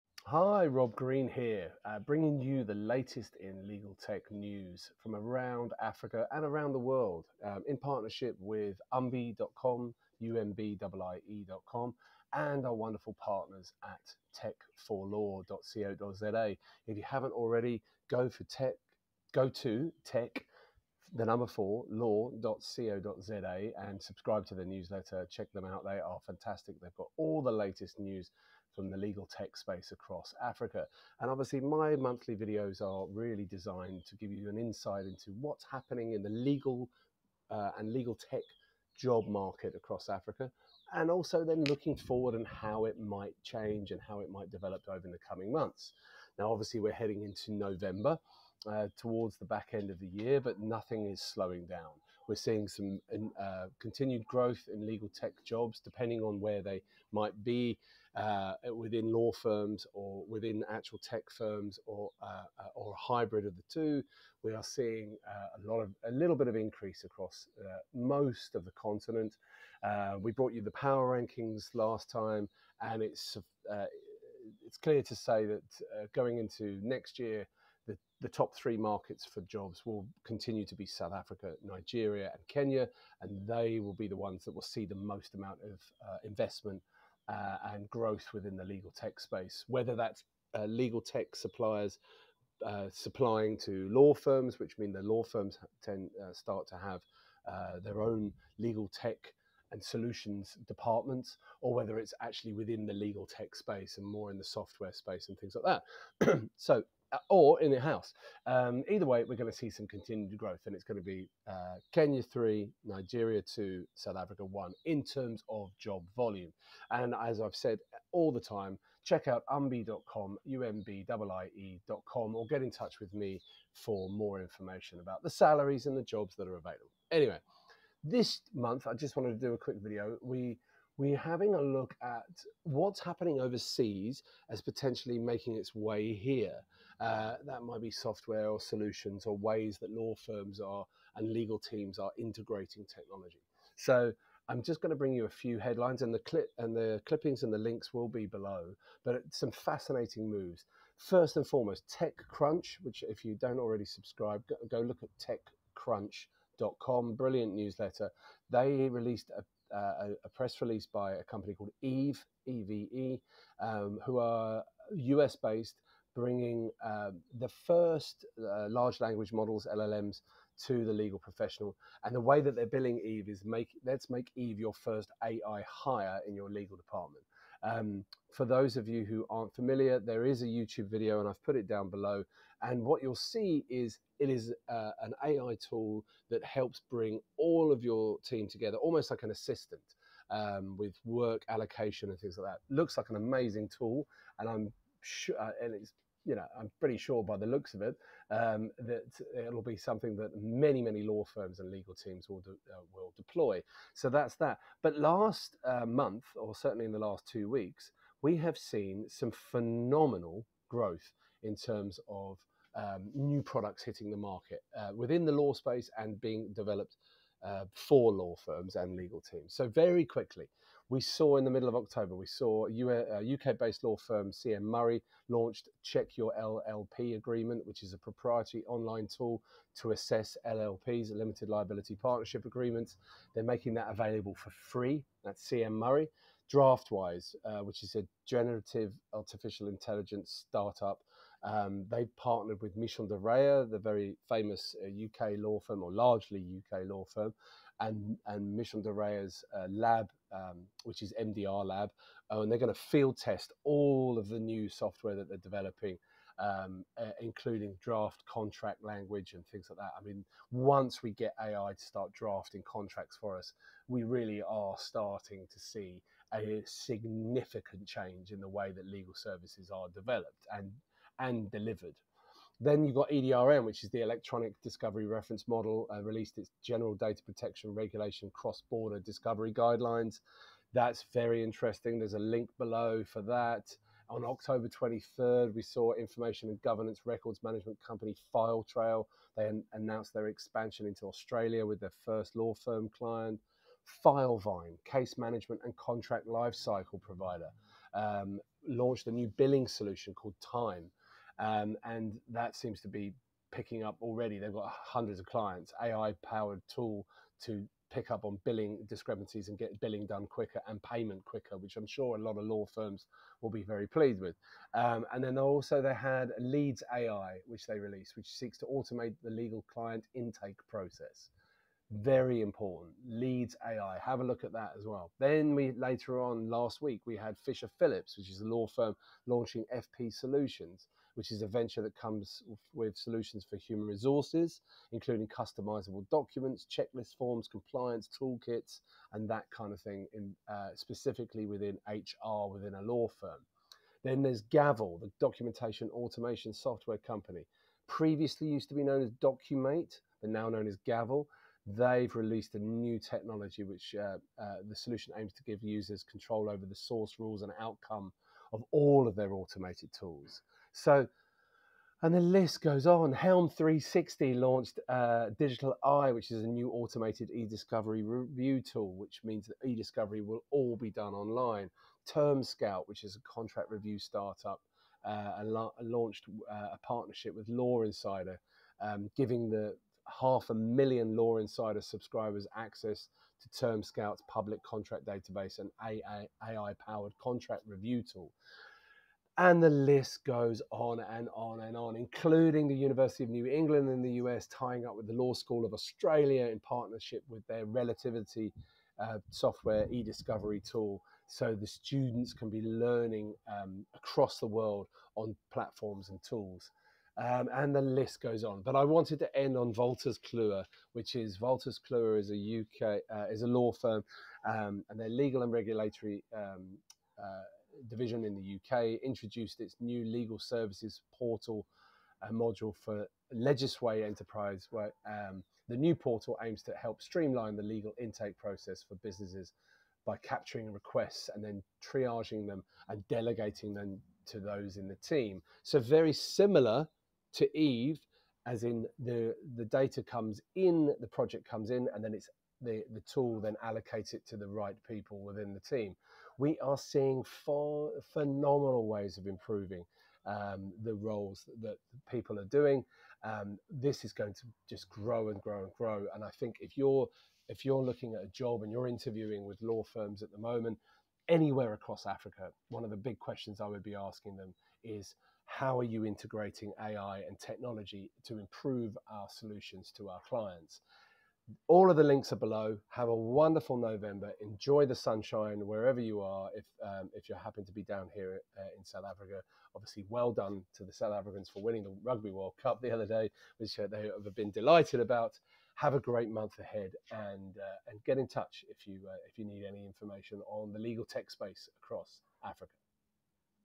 Stay Ahead of the Game: AI Legal Jobs in Africa | Monthly LegalTech Update
(TechCrunch) Eve launches to bring LLMs to the legal profession

Legaltech News brought us a great run-down the other day.

00:00 Intro
00:50 Continued job growth
01:10 Power Rankings
02:10 What is happening overseas, that might get to Africa?
04:00 Unbelievable tools being released